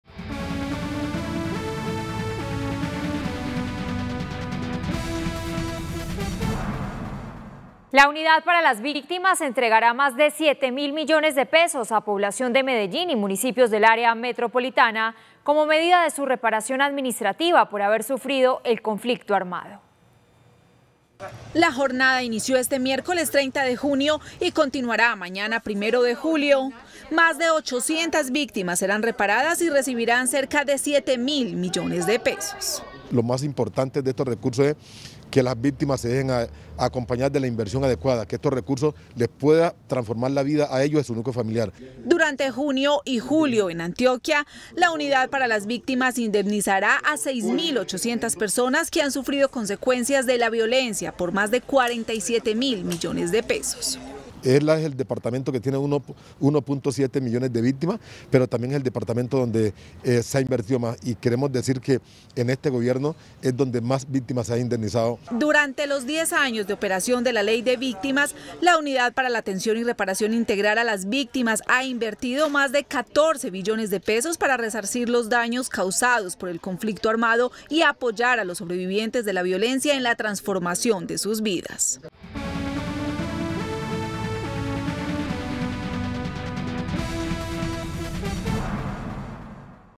Otras 865 víctimas del conflicto recibieron reparación - Teleantioquia Noticias
La Unidad para las Víctimas entregará más de 7 mil millones de pesos a población de Medellín y municipios del área metropolitana cómo medida de su reparación administrativa por haber sufrido el conflicto armado.